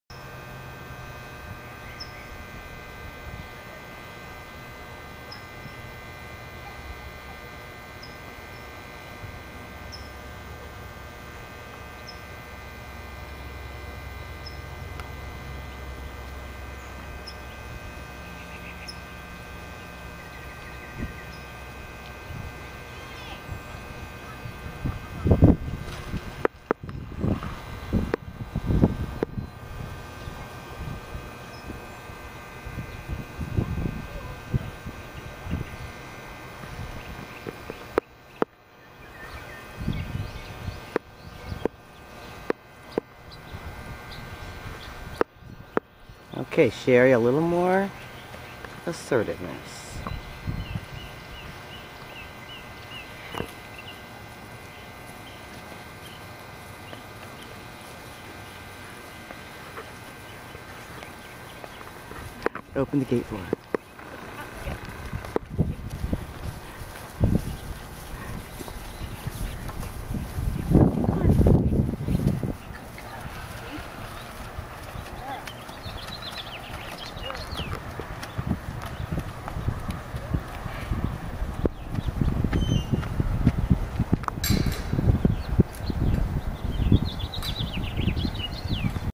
Jenny walking to her pasture
This is Sheri using all her brave, new courage to lead Jenny from her night-time area to her day pasture.